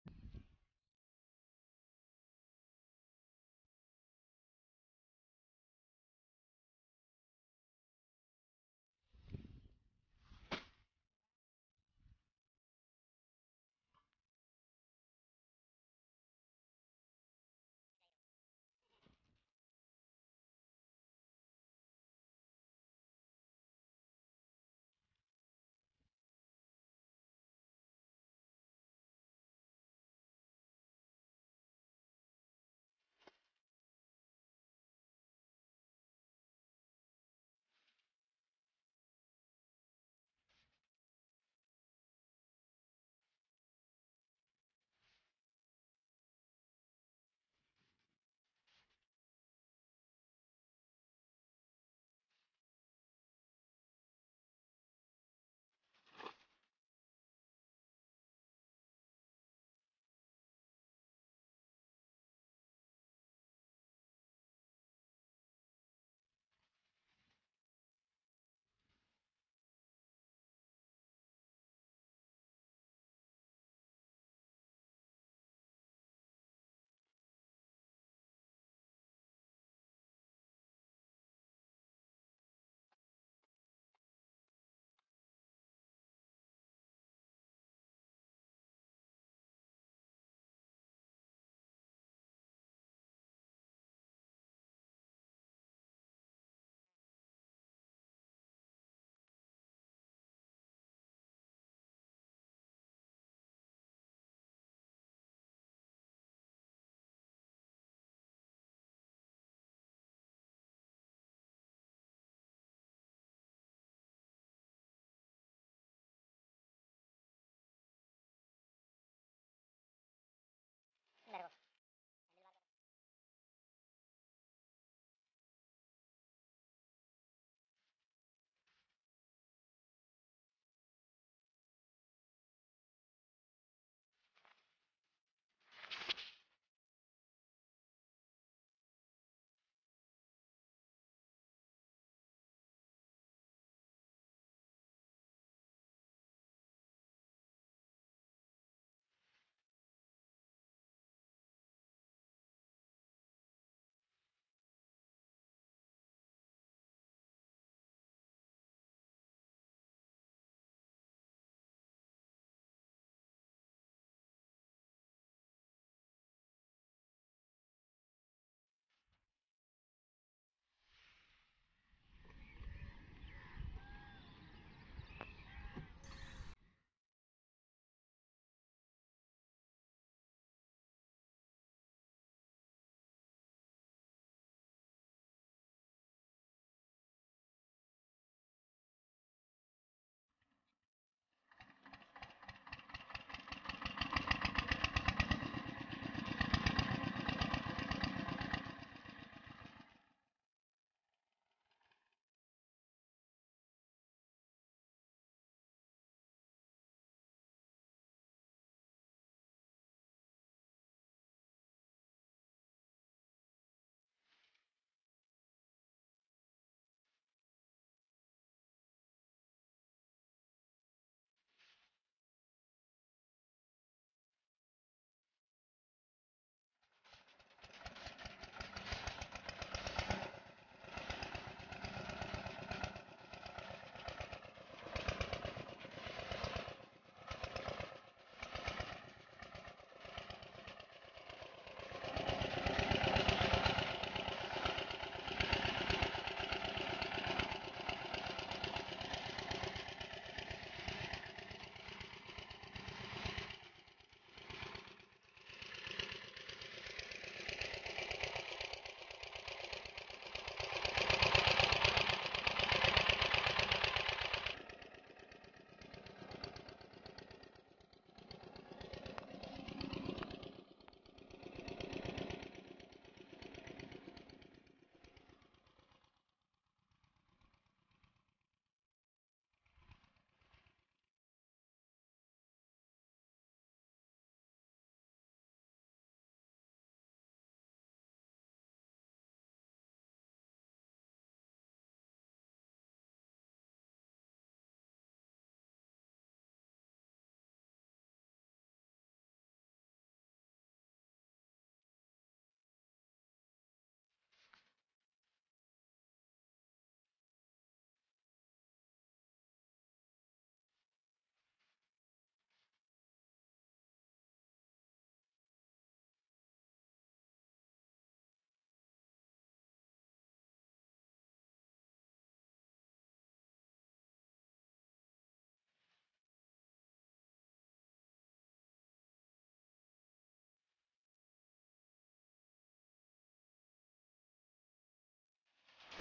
4 inch 500 Miter plastic pipe and 8hp diesel engine water pump sinchai working
4 inch 500 Miter plastic pipe and 8hp diesel engine water pump sinchai working